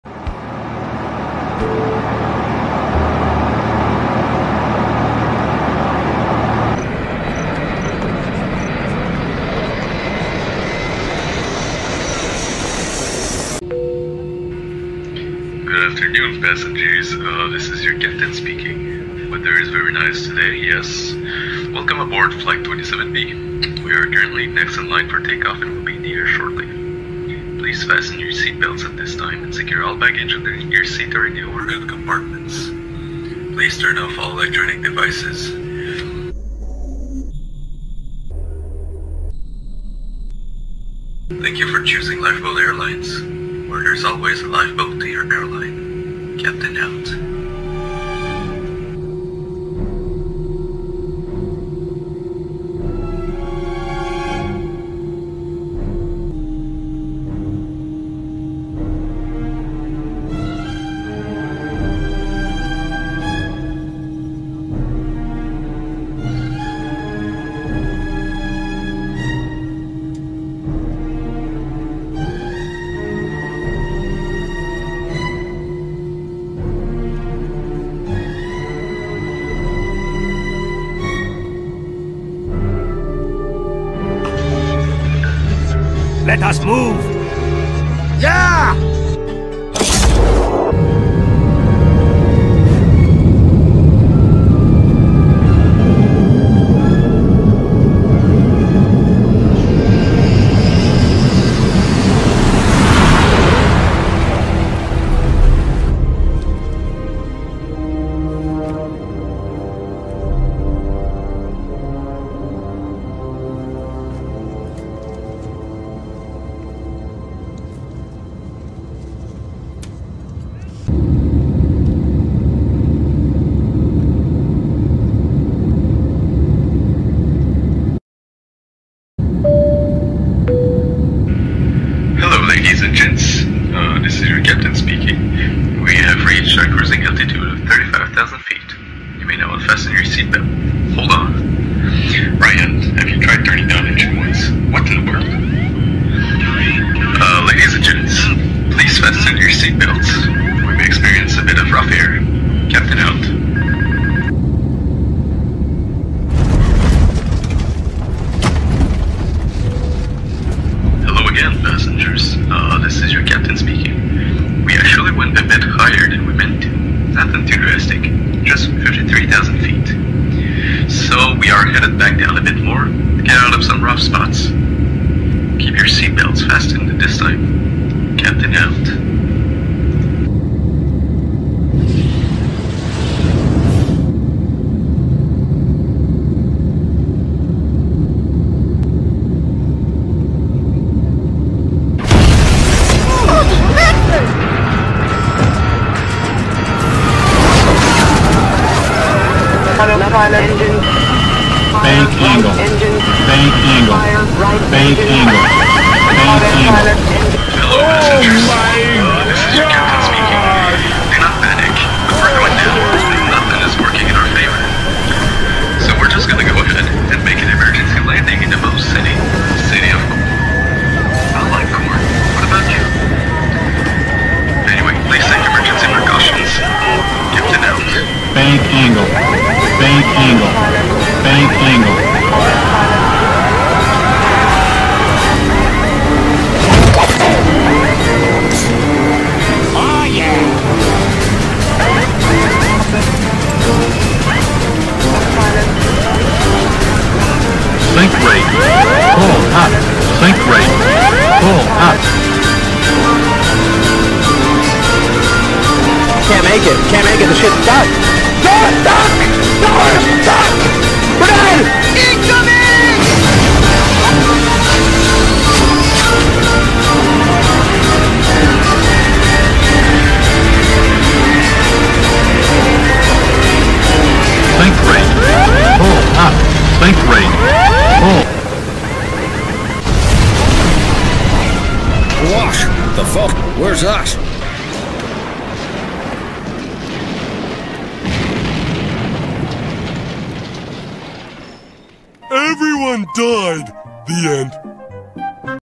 Lifeboat Airlines Flight 27B with Chaos
audio credits by totally not random stuff xd
I recreated the totally not random stuff xd lifeboat airlines flight 27B